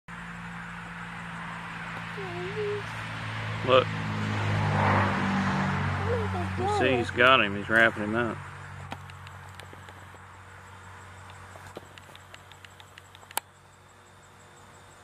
Garden Spider vs Moth
A garden spider wrapping up a moth for later. Argiope aurantia